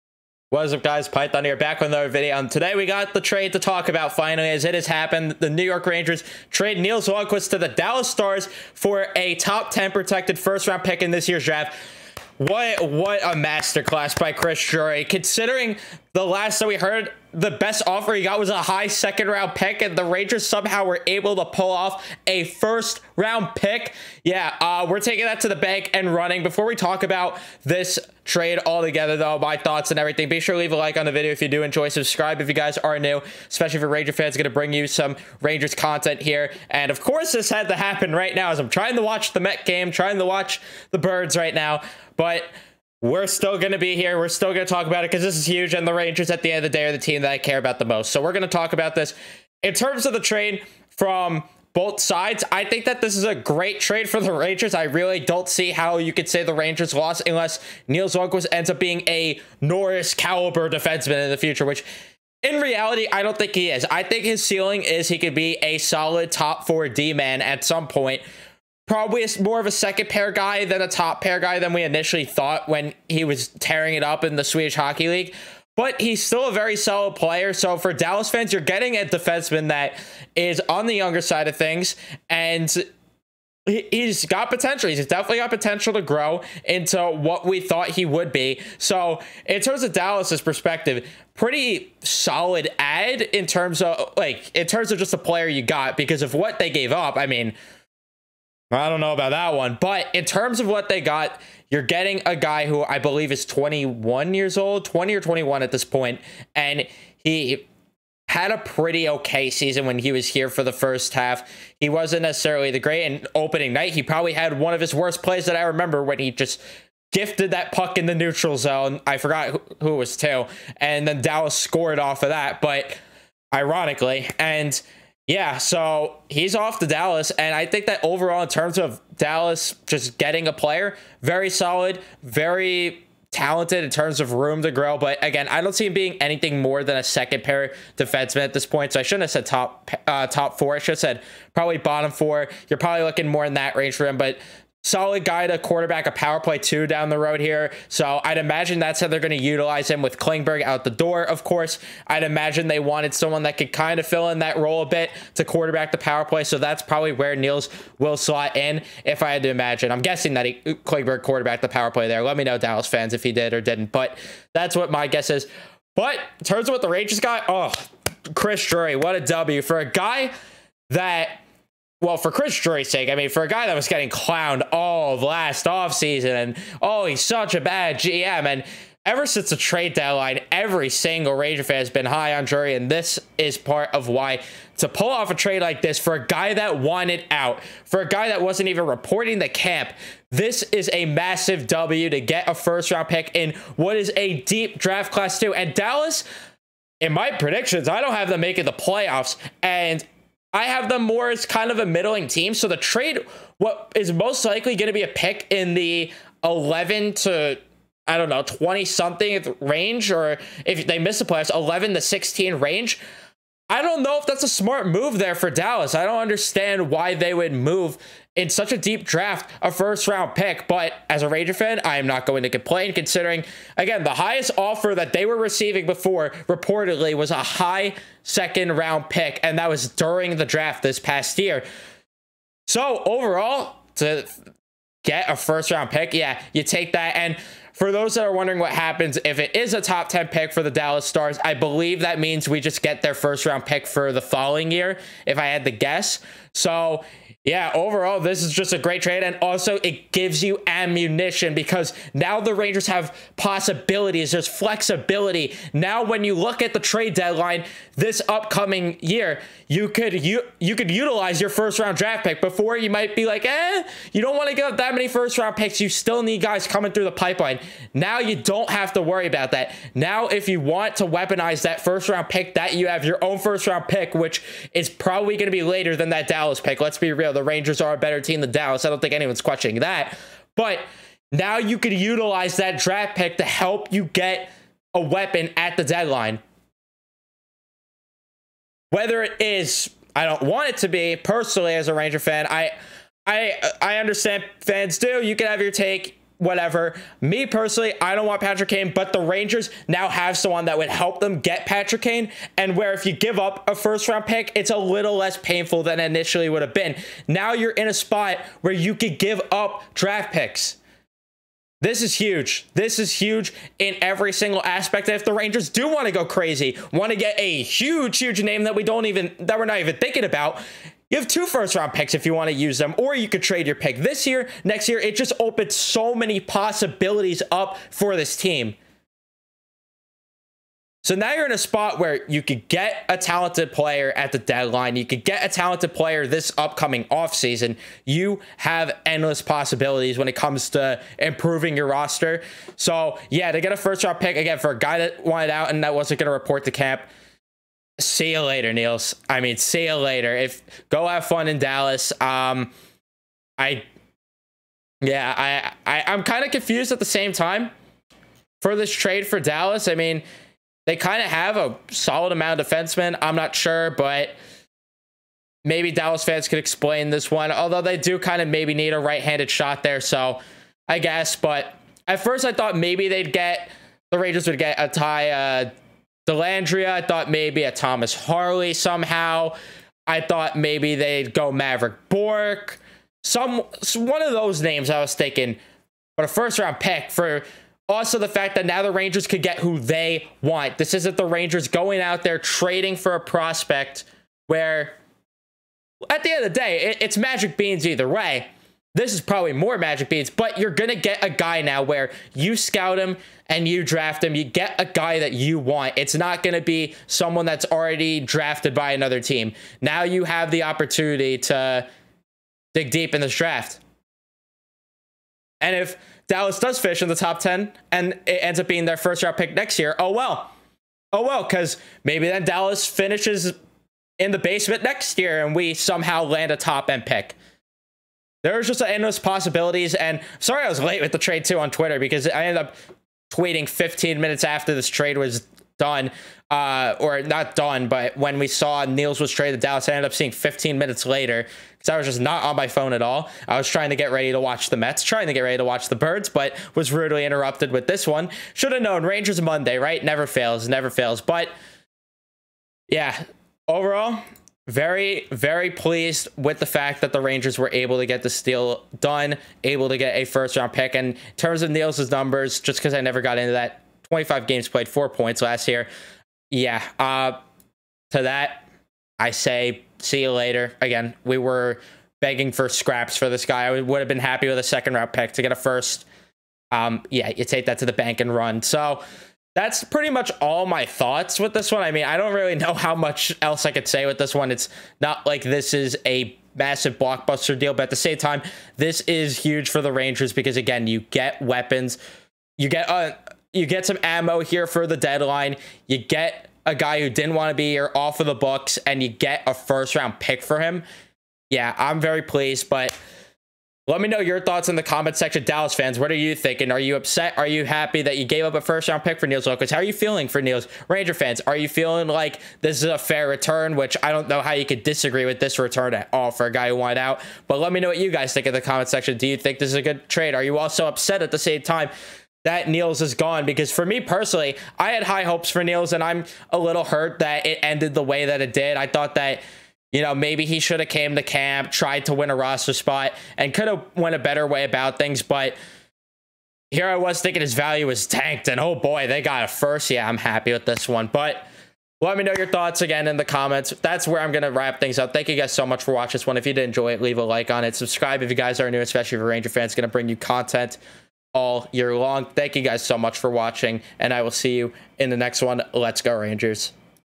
THE NEW YORK RANGERS TRADE NILS LUNDKVIST FOR A 1ST ROUND PICK! My Thoughts & Reaction!
Get Yourself To A Game By Using This Link To Tickpick

Call in whenever to be featured in a video: (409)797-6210